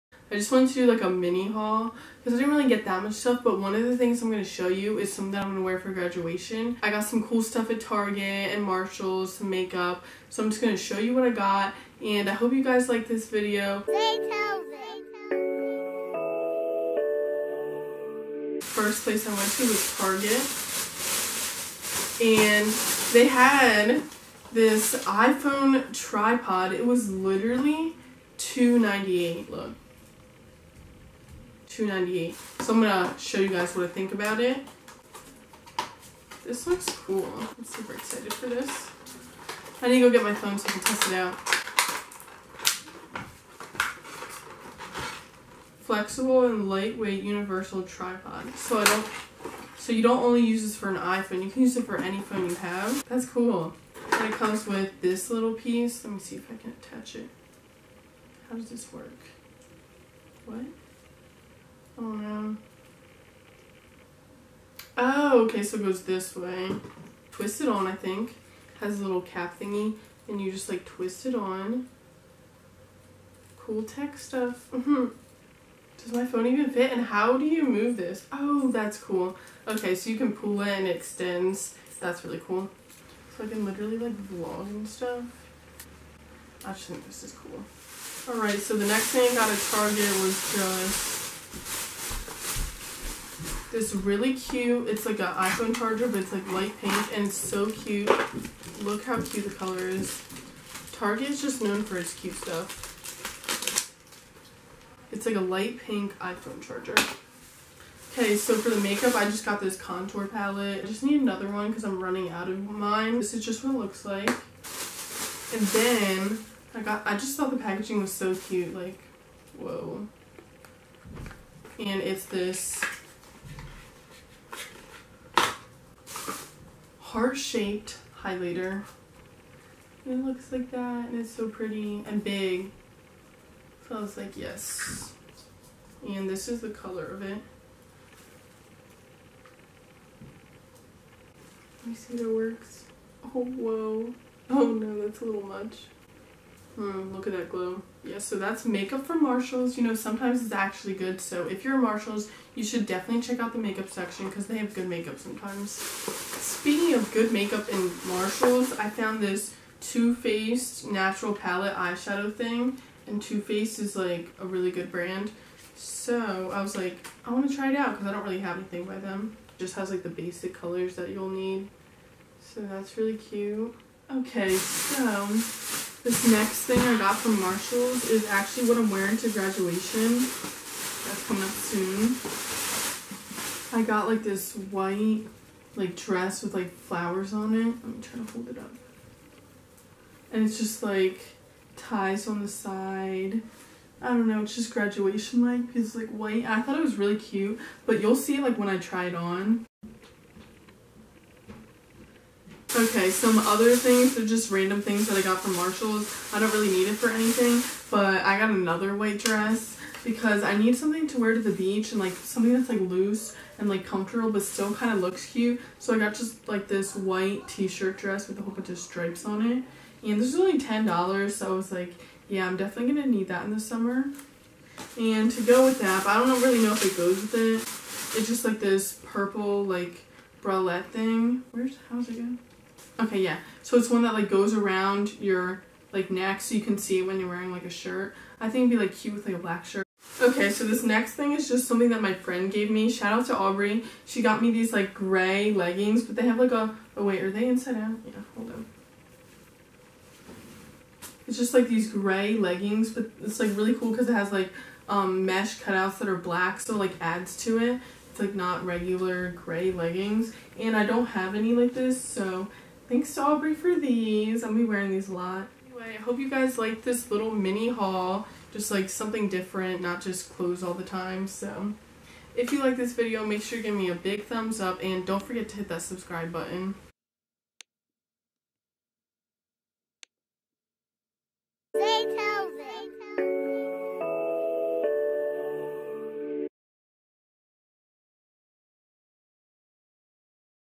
TARGET CLEARANCE HAUL!! (and Marshalls TRY-ON)
Just decided to do a quick little mini haul, show you guys what I got from target and Marshalls.